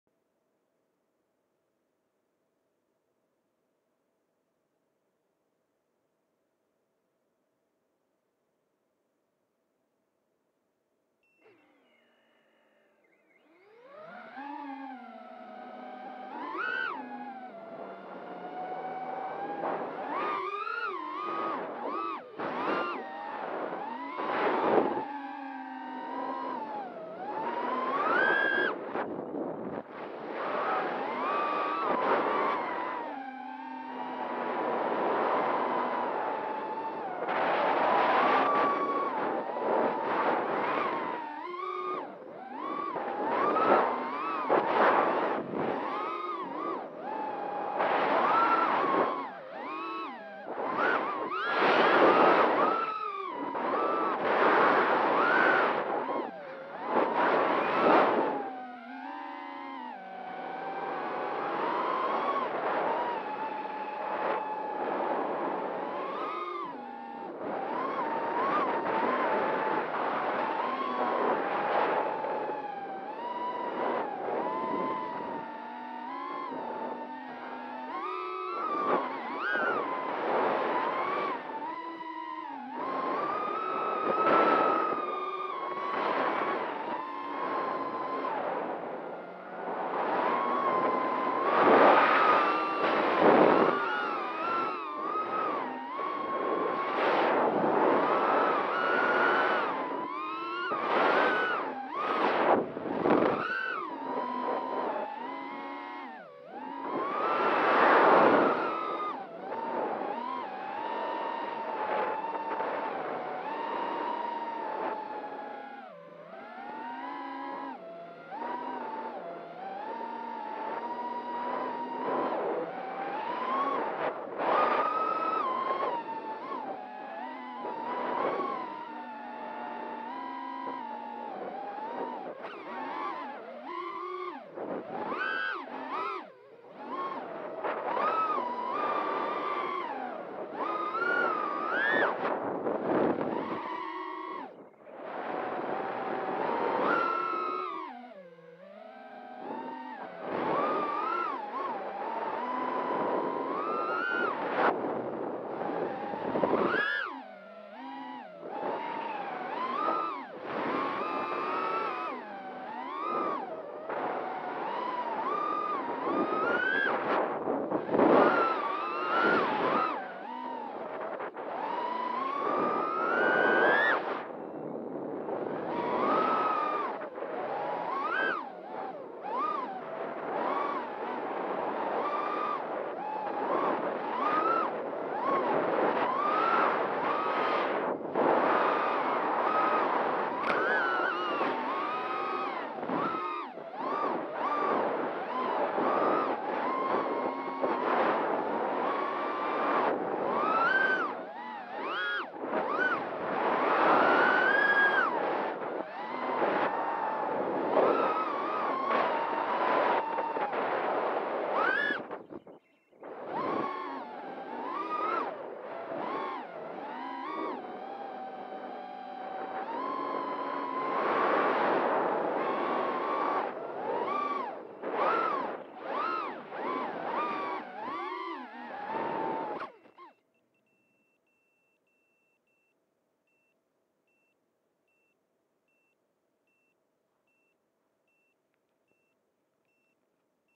learning how to acro day 2 first power loop part 4
did  a scary power loop I got scared cause it was kinda close to me but doesnt look so close when I rewatched it few more tricks